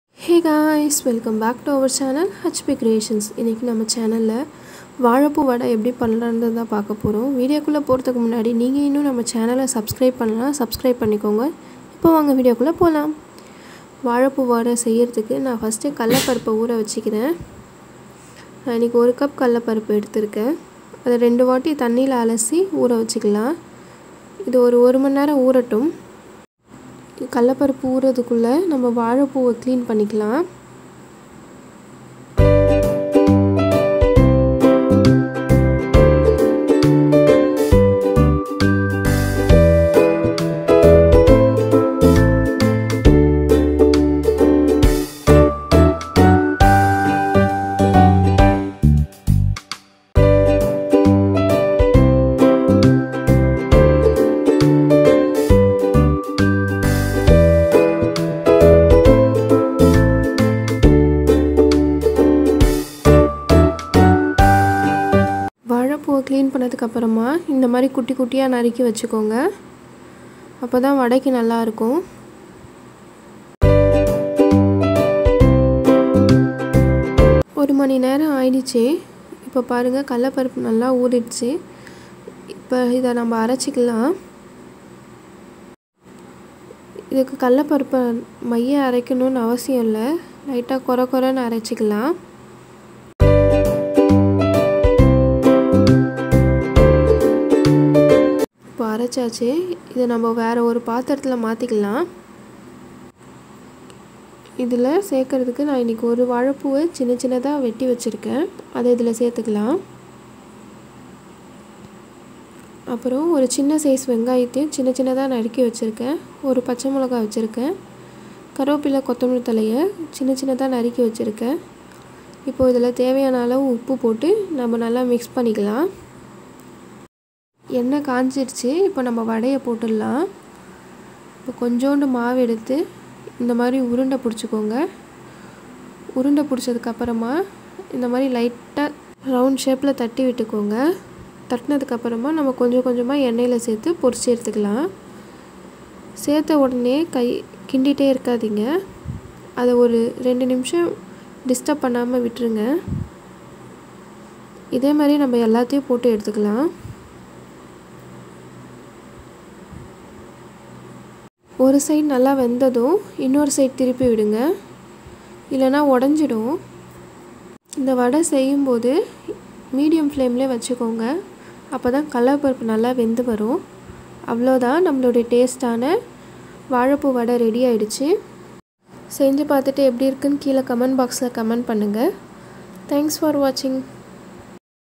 vazhaipoo vadai | வாழைப்பூ வடை | hp creations
This video is about how to make vazhaipoo vadai
Thanks for ur love and support guys